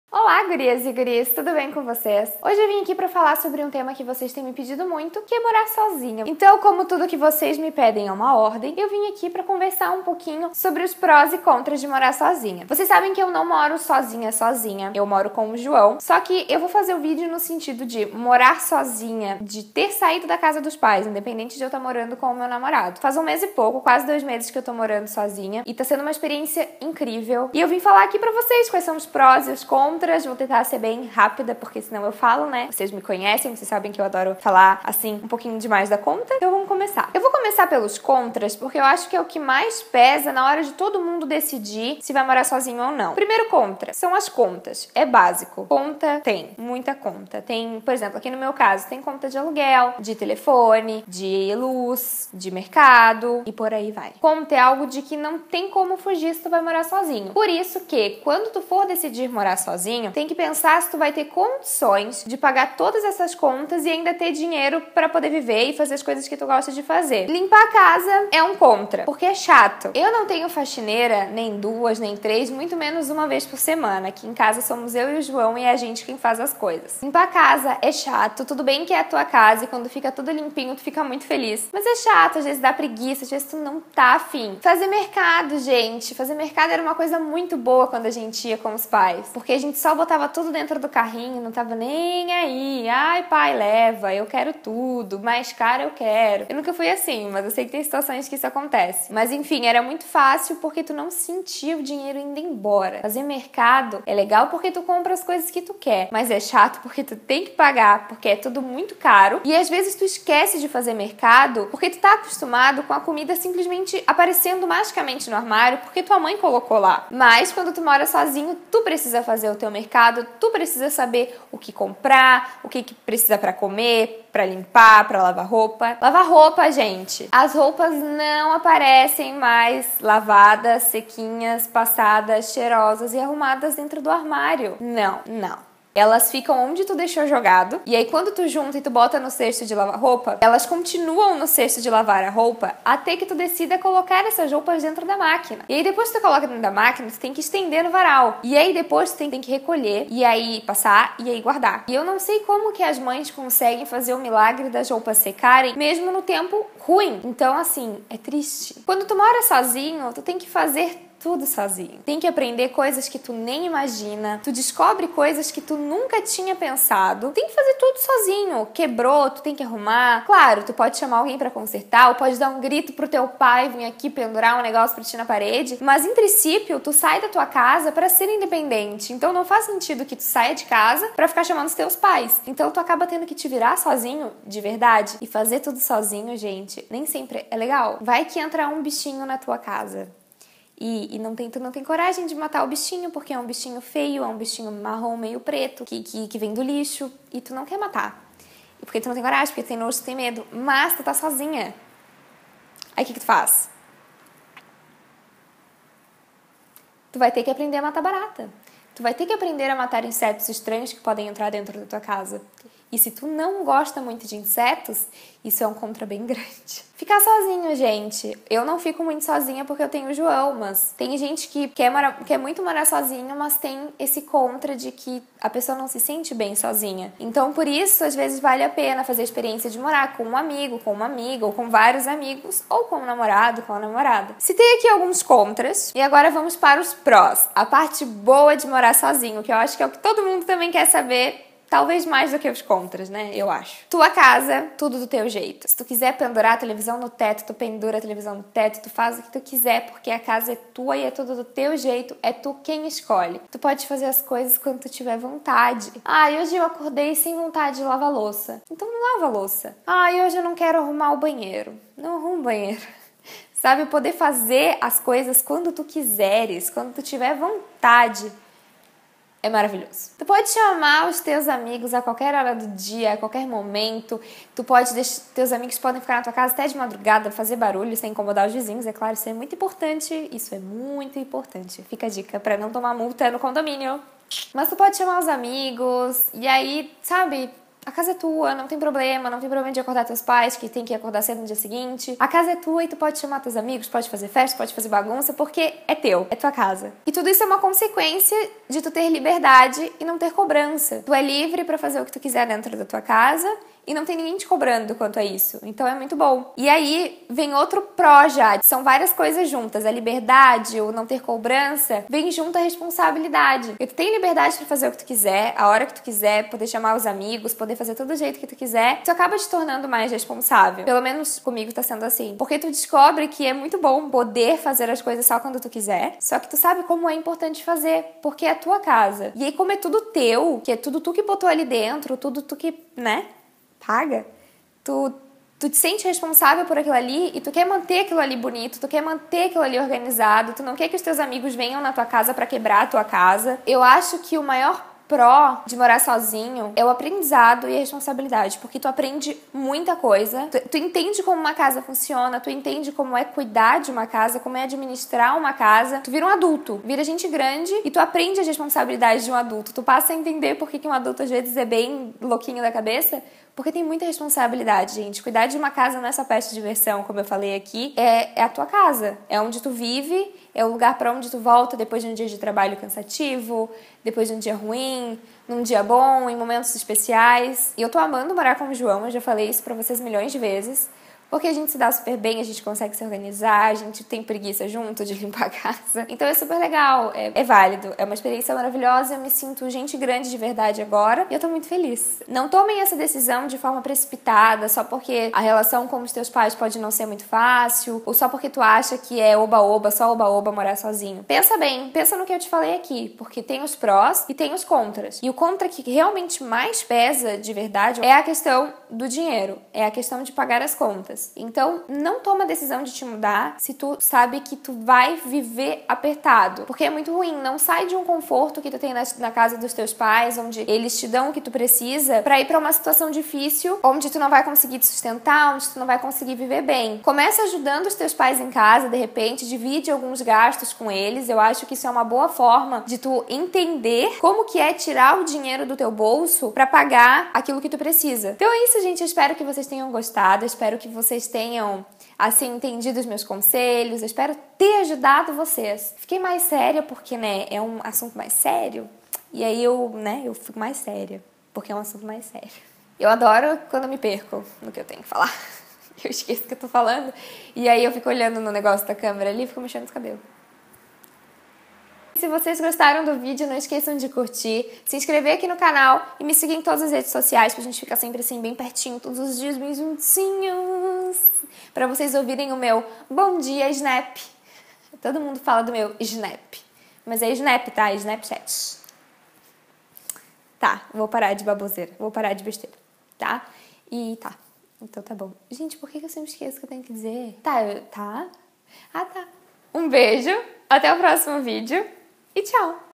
PRÓS E CONTRAS DE MORAR SOZINHA
Vocês me pediram muito um vídeo falando sobre os prós e contras de morar sozinha. Qual o lado bom e o lado ruim de sair da casa dos pais pra começar a vida na sua própria casa? Espero que gostem, se divirtam e me contem nos comentários qual a visão de vocês sobre o assunto. Amo vocês! :)

☆ periscope: blogvanessalino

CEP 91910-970

Se gostar do vídeo, curta e compartilhe! =)